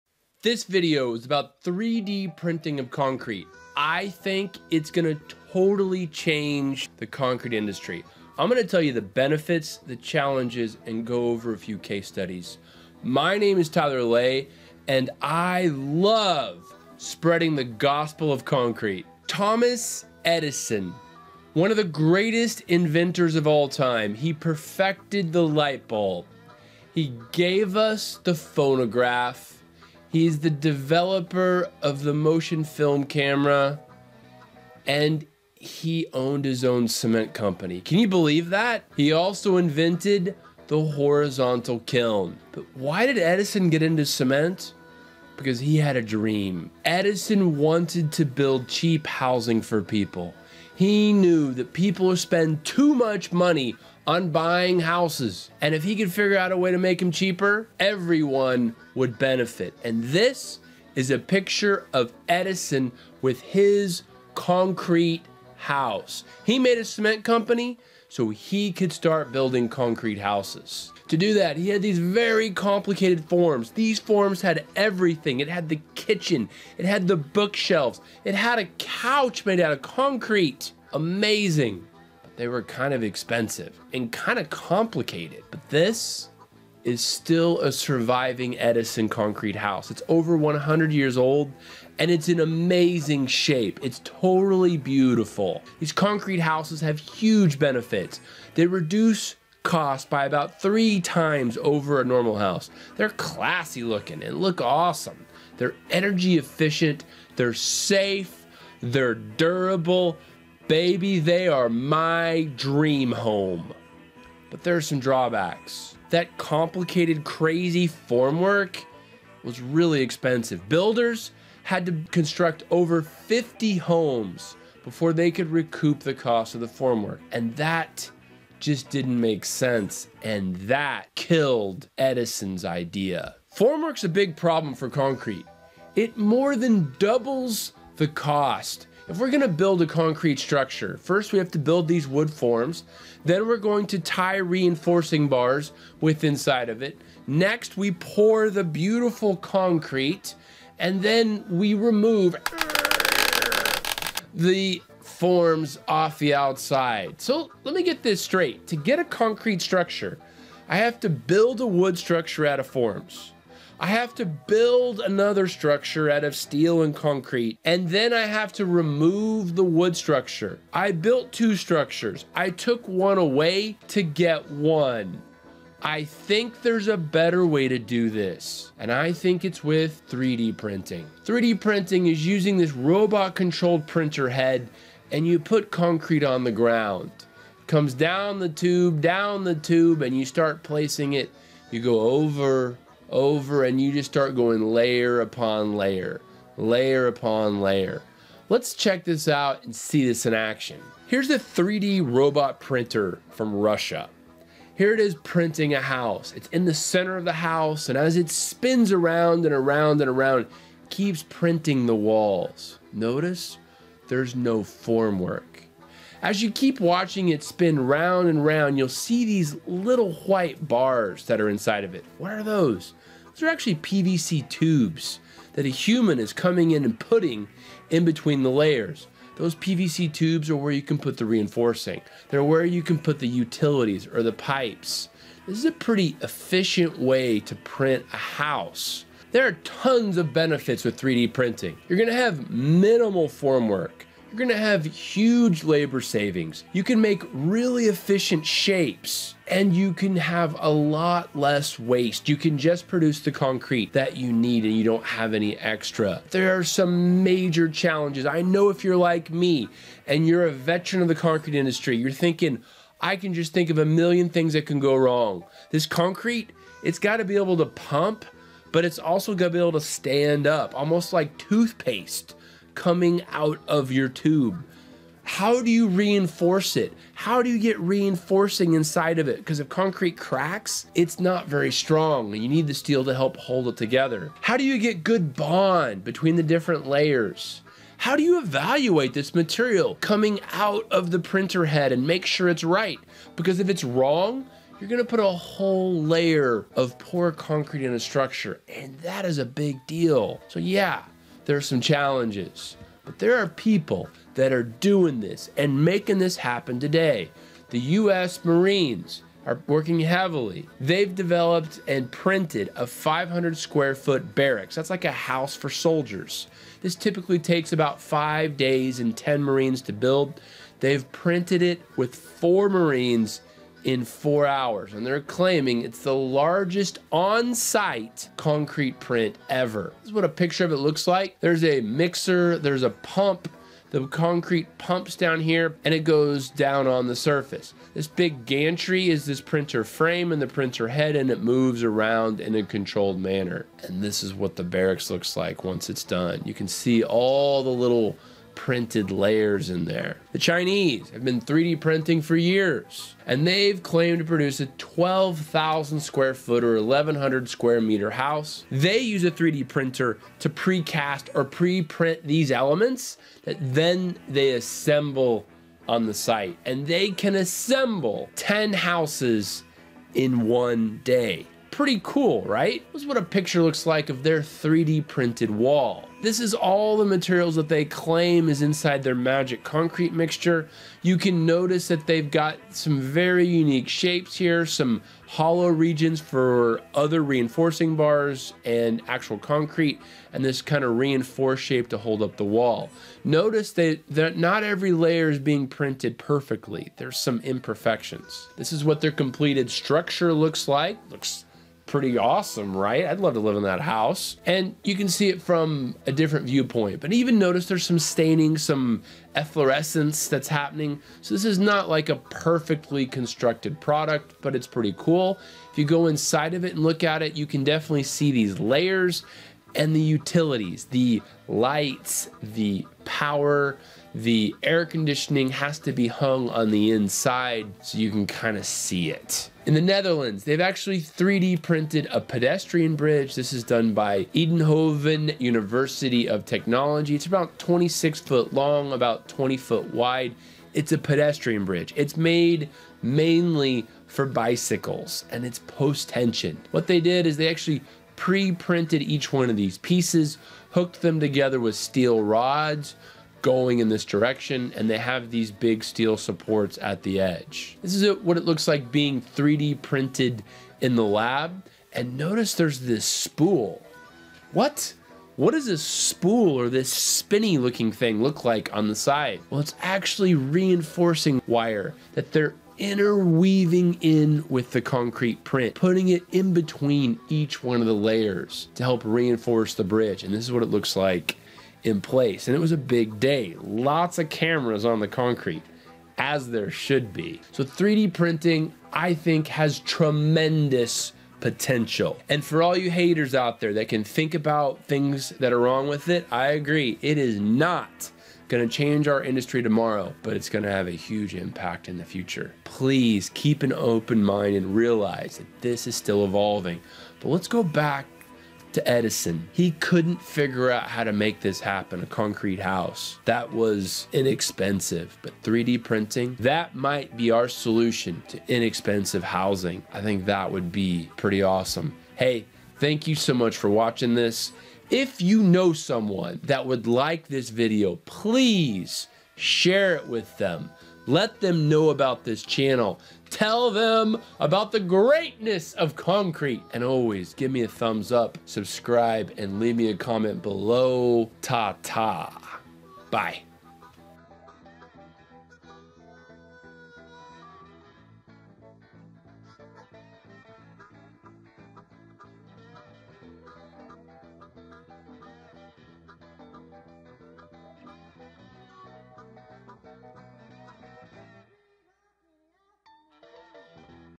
3D Printing – A concrete revolution!!!
Additive manufacturing or 3D printing is transforming many industries.  This video explains why 3D printing will revolutionize certain types of concrete construction.  A discussion is also included over the challenges that face 3D printing.  Finally, several 3D printing case studies will be presented that show how homes and bridges are being constructed with 3D printing.

Thanks to Hailey Goodale!  She edited this video and is my Director of Awesome.